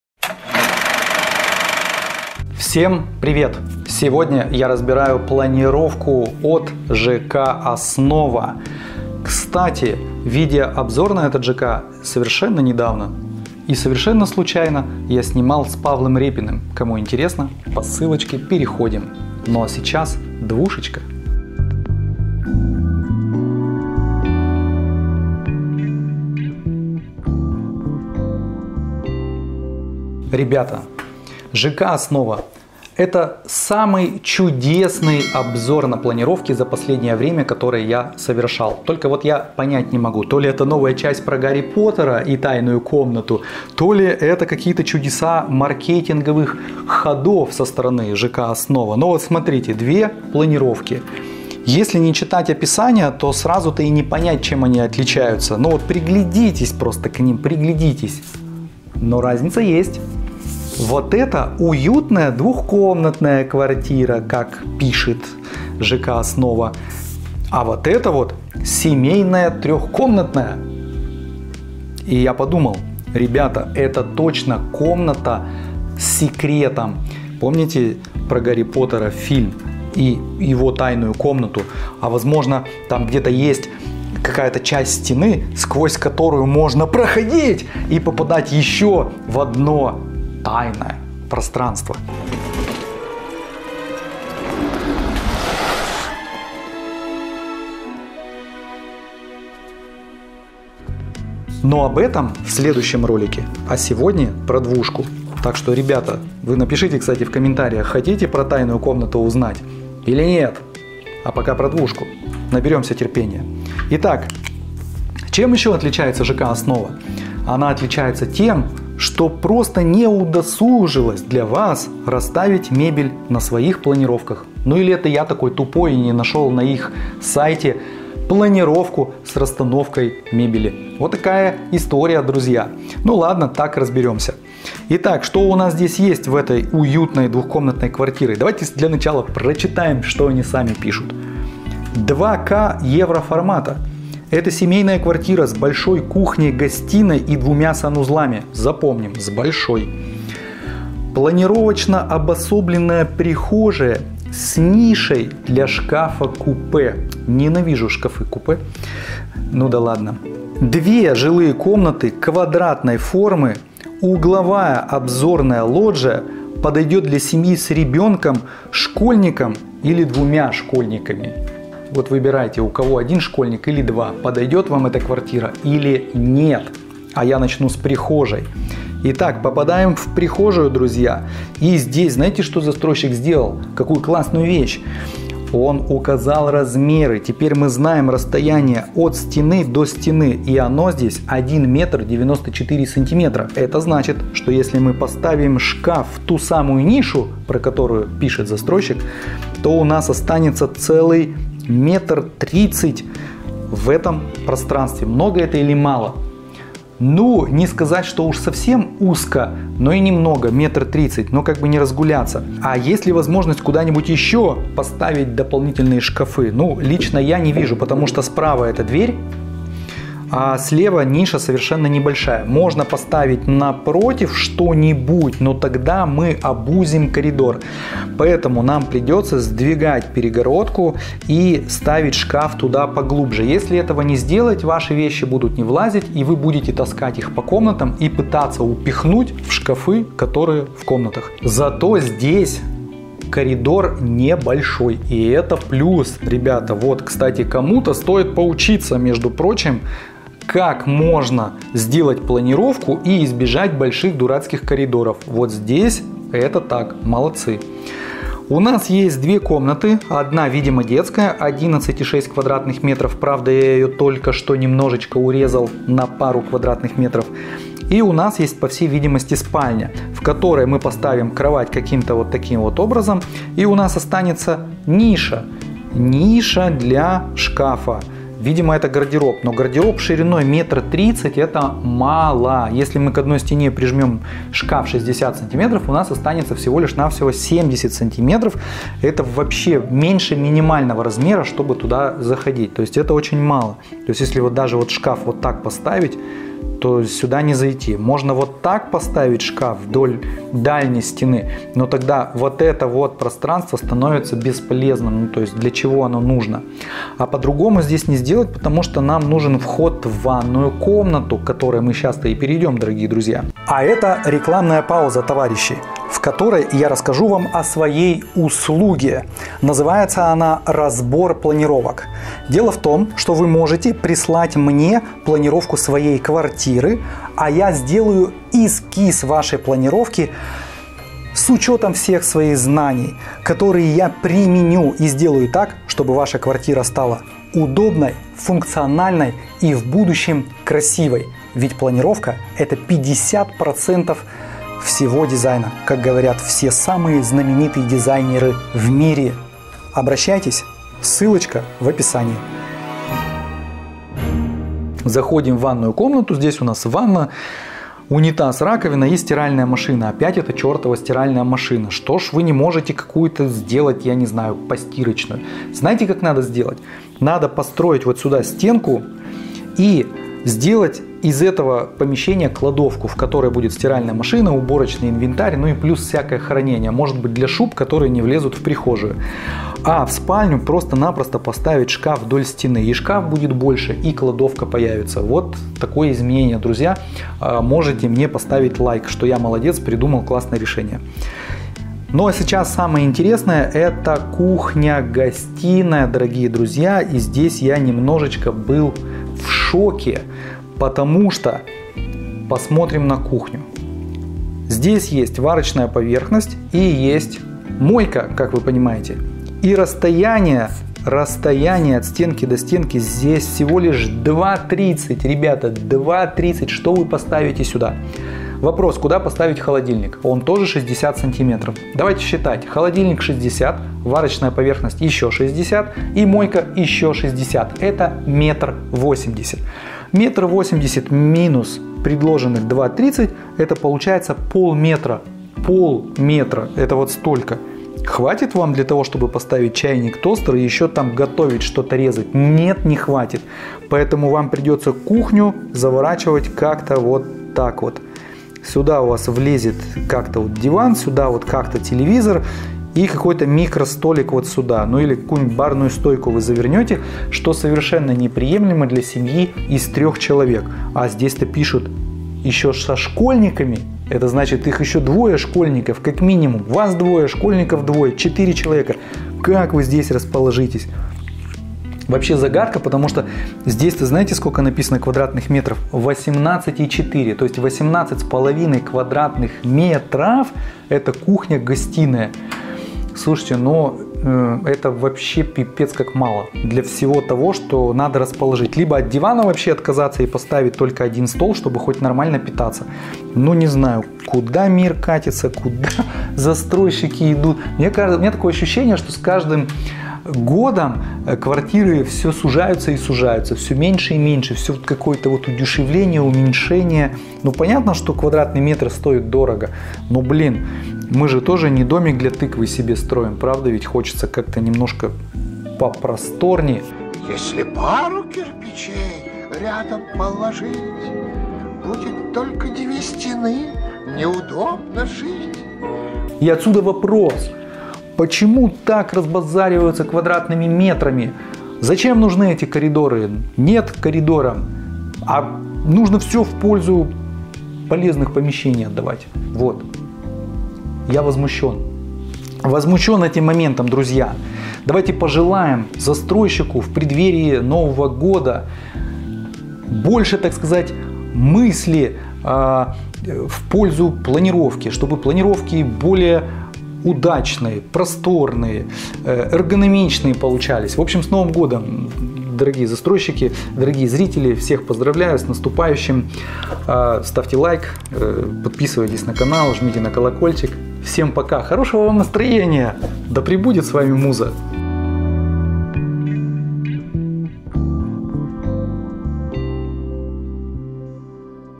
НЕ ДЕЛАЙТЕ такую кухню-гостиную! Разбираем планировку двушки в ЖК Основа. Новостройки Екатеринбург
Приветствую на канале! Я Иван Безруков, дизайнер и руководитель студии Градиз интерьер. Сегодня мы разбираем планировку двушки от ЖК Основа Екатеринбург. В программе квартира с кухней-гостиной, не приспособленной к адекватной эксплуатации.

Застройщик Фортис Екатеринбург предложил не лучшую, как по мне, планировку для двушки. Двухкомнатная квартира в ЖК Основа предлагает нам прихожую с местом под шкаф-купе, небольшие коридоры, две спальни, а также кухню-гостиную. Если вы решили купить недвижимость, обязательно посмотрите это видео, так как я дам несколько советов по перепланировке.

Подробнее в видео:
0:00 Обзор новостроек.
2:57 Прихожая.
4:00 Коридор, детская.
4:29 Родительская спальня.
5:56 Ванная.
6:37 Кухня-гостиная.